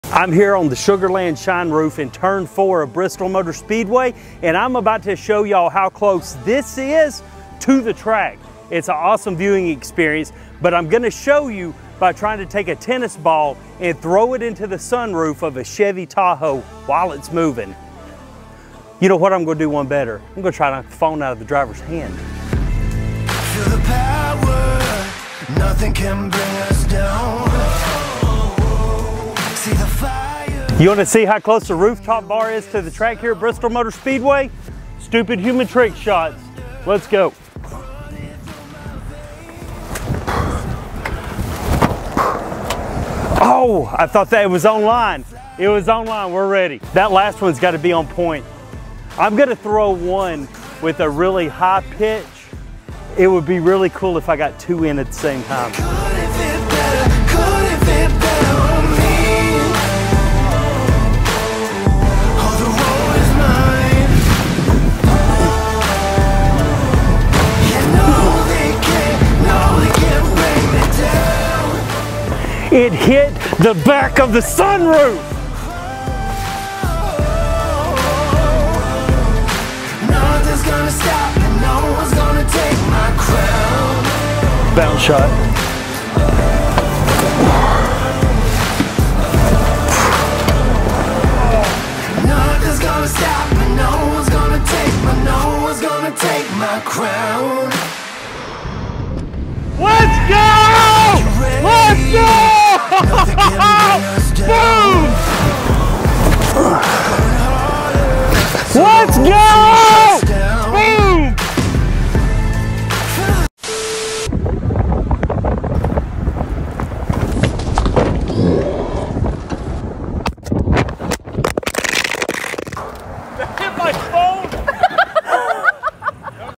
Trick Shots at Bristol Motor Speedway
Think I can sink a tennis ball into the sunroof of a moving @Chevrolet Tahoe from the ​⁠Sugarlands Shine Roof in turn 4 of @ItsBristolBaby